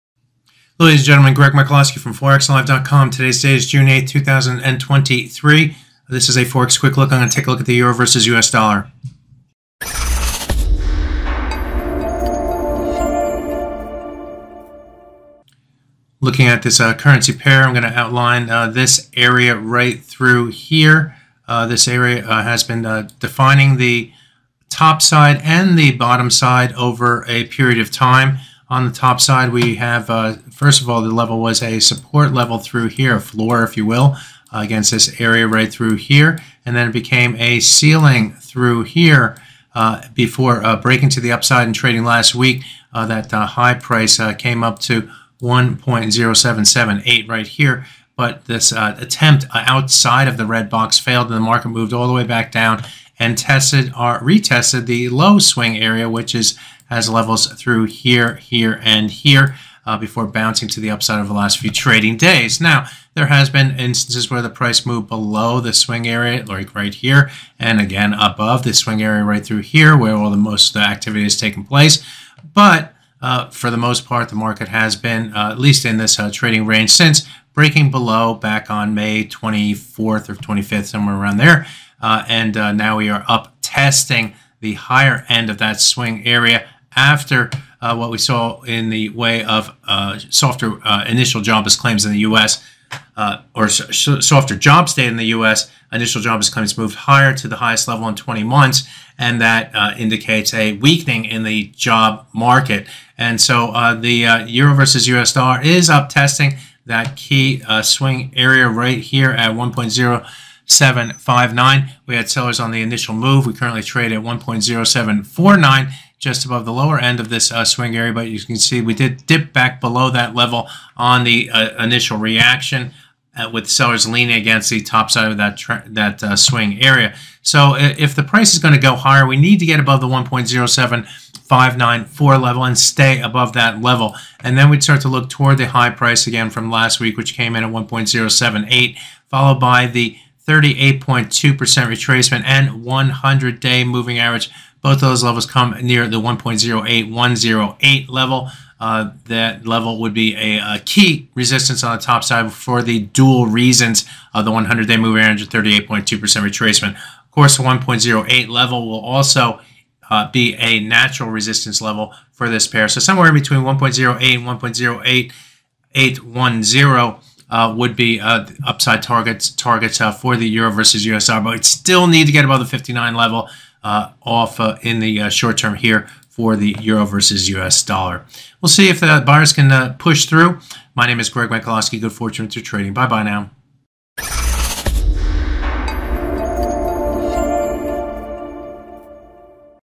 EURUSD runs to swing area after weaker jobs data
Initial jobs clients spikes to 20 month highs helping to weaken the USD.

The initial jobless claims for the current week rose to the highest level in 20 months and that has weakened the US dollar in the process.  Looking at the EURUSD, the price action saw the pair move up to a swing area between 1.0747 and 1.0759, but is finding sellers against that swing area on the first look as risk for sellers can be defined and limited by sellers.  A move above would open the door for a run toward last week's highs at 1.0778. Move above that and traders would start to target the 38.2% retracement of the major trading range, and the 100-day up at 1.08108 (with the natural resistance at 1.0800 in the way). Earlier today the EU GDP moved into negative territory at -0.1% versus 0.0% expected for the 1st quarter growth. That may somewhat limit the upside, although the price of the EURUSD has been moving to the upside and away from its 100/200 hour moving averages in trading today. The hurdle remains the 1.07594 level. Get above, and stay above, and more probing toward 1.0800 to 1.0810 can be anticipated.